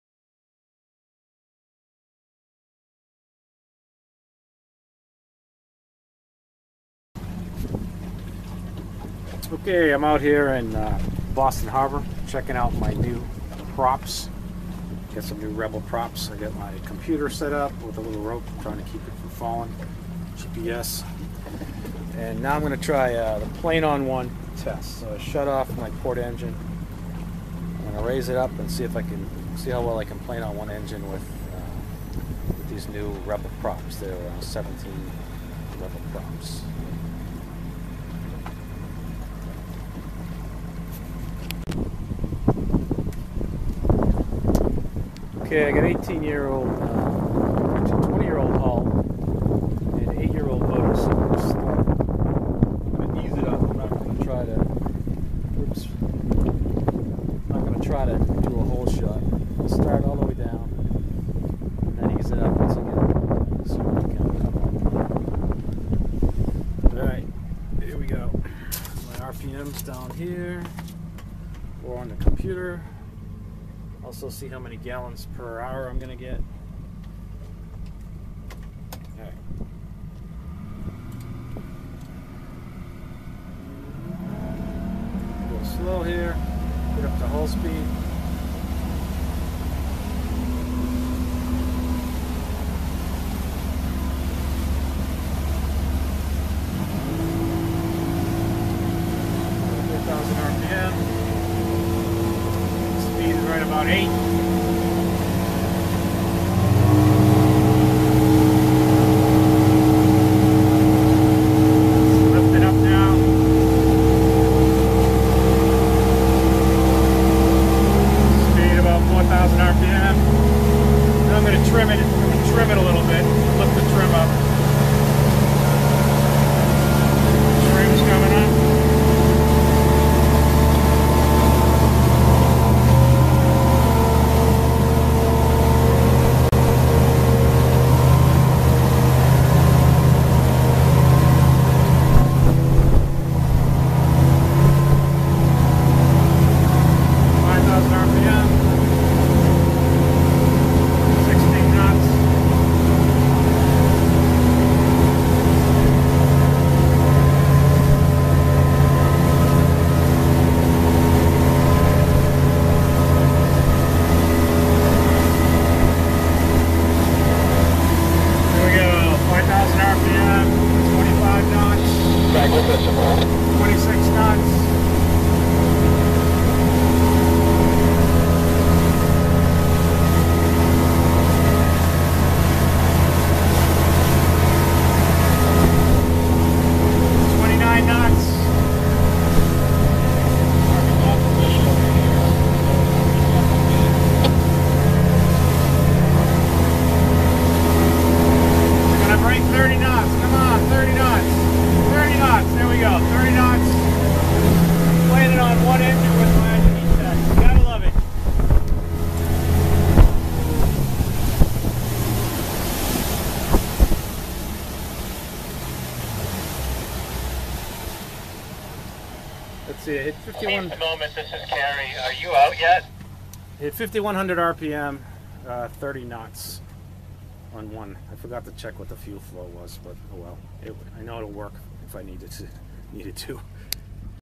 Plane-on-one test with 17" Rebel Prop on E-TEC 200HO for Grady White Sailfish
I replaced my Viper 19" props with Rebel 17" props and did a number of tests to compare performance. This test is a "plane on one" test where the boat gets up to ~35 mph using a single outboard running at 5100 RPM. Normal full-throttle with both engines is 5700 RPM and 49 MPH.

The audio about 1 minute in is difficult is understand but what I am saying is that with a 1994 Hull/Transom and near 8 year old E-Tec engines, I'm not going to try a hole shot - rather, I'm going to gradually increase the throttle until eventually reaching WOT. You can hear me inching it up at 1:37 and again at 1:47 where it starts to climb up onto plane  and hits 13 kts (15mph) at 2:03 and 4000 RPM. I start messing with the trim at this point and end up at 1/4  trim at 2:48. During the time I was messing with the trim I inched the throttle up the rest of the way. The final number were 30.1 kts (~35mph) at 5100RPM. The actual trim tabs were up and out of the way - all trim was handled with the single outboard. I probably could have got slightly better performance if I used them as well but was trying to keep it simple.

The audio at the end that is difficult to understand is me commenting on the fact that I forgot to check the fuel burn rate. The normal WOT fuel burn rate is 21GPH per engine so it was probably close to that.

Note that this mid 90's Sailfish is not known for great planing performance because it has a shortened running surface compared to the 1998 and later models where the  running surface extends to the end of the Euro-transom. Thus this should be easier to do on the later Sailfish including both the 272 and 282 (which are the same length.)  There is a typo in the video where I list the beam as 9'8" - the correct beam is 9'6".

I hope I never have to come back on one engine but it's good to know that I can do it in a hurry if I need to.

Cheers,
-Scott